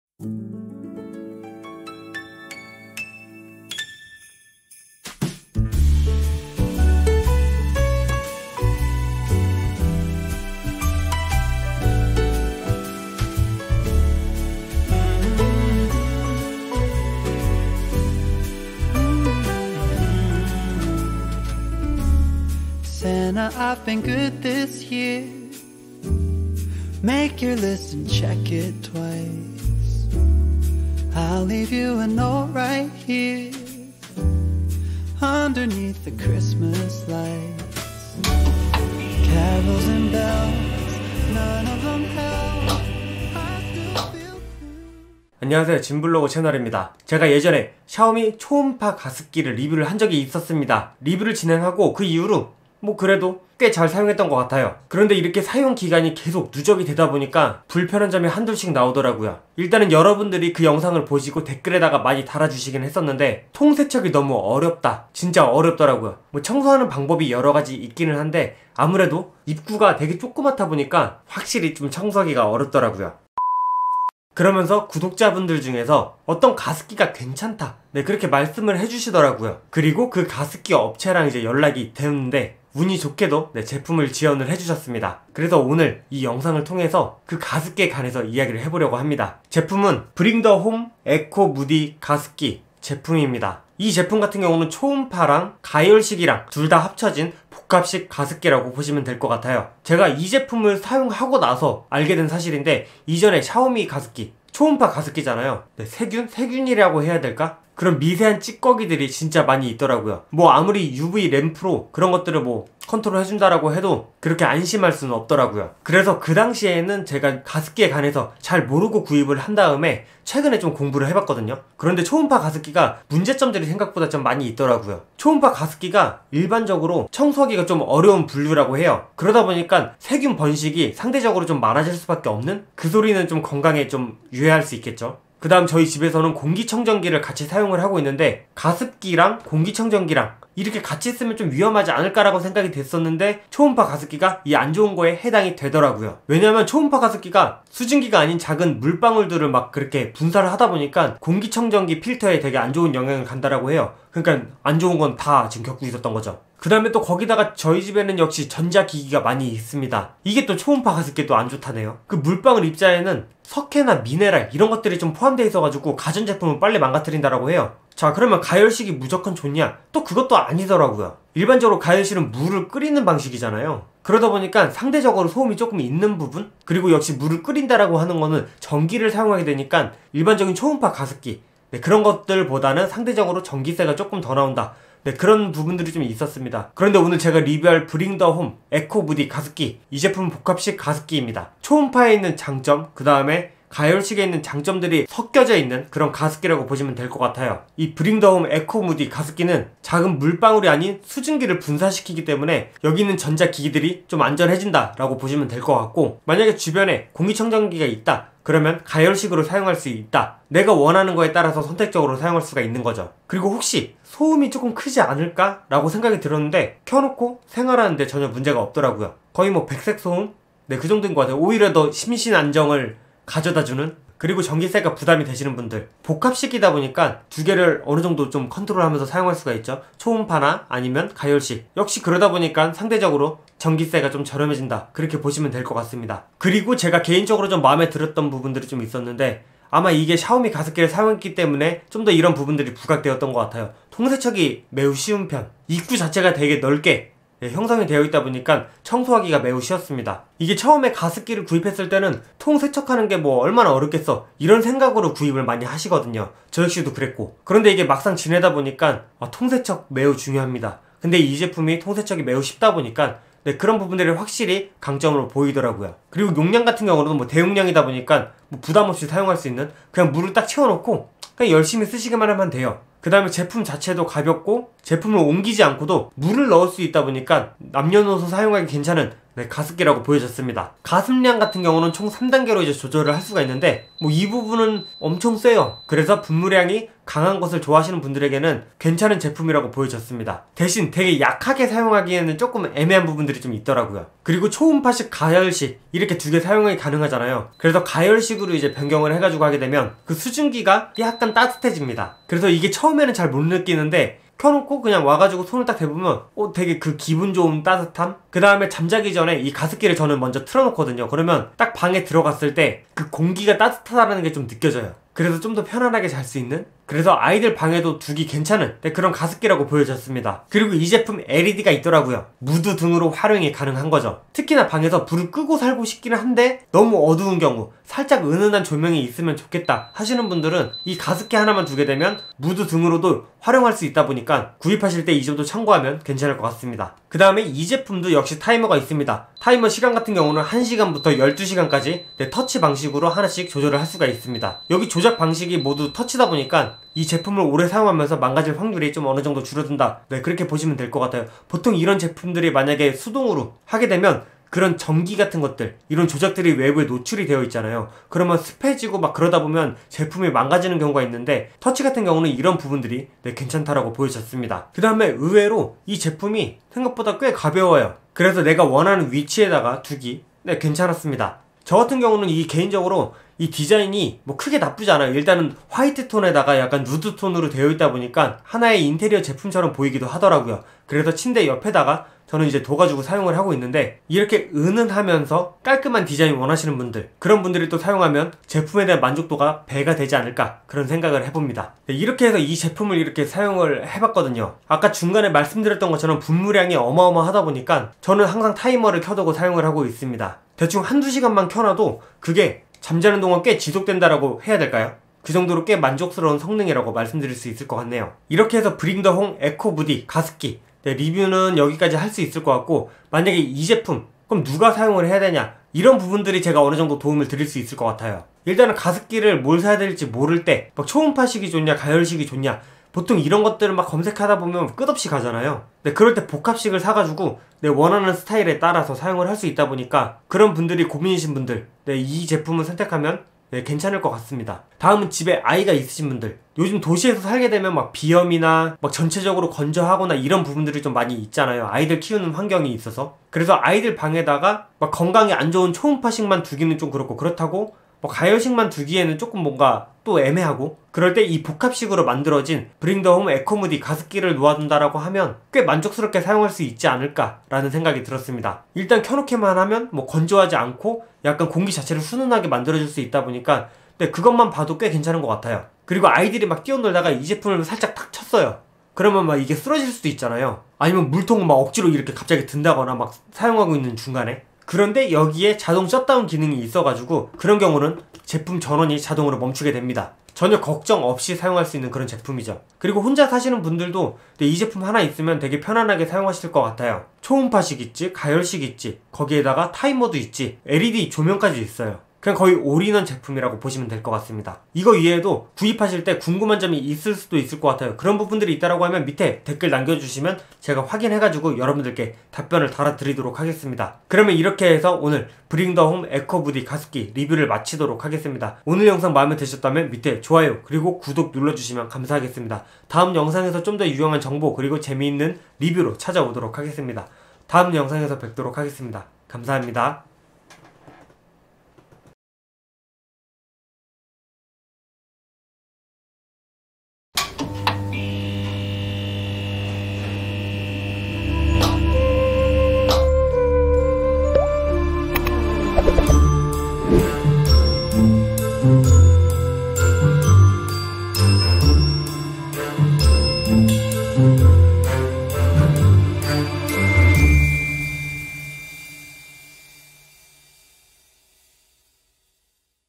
🌊 [리뷰] 브링더홈 복합식 대용량 에코무디 가습기 사용기 리뷰 | 4L LED 무드등 초음파 가열식 저소음 분무량 타이머
"본 영상은 브링더홈으로부터 제품을 무상으로 제공받았으나 저의 주관적인 의견을 바탕으로 제작되었습니다."

과거 가습기 구입 후 실패를 맛본 뒤, 제대로된 가습기를 가지고 왔습니다!
올인원 가습기라고도 말씀드릴 수 있을 정도로 모든 것을 다 갖춘 복합식 대용량 가습기입니다.
가습기의 종류와 브링더홈 복합식 대용량 에코무디 가습기를 같이 알려드리겠습니다!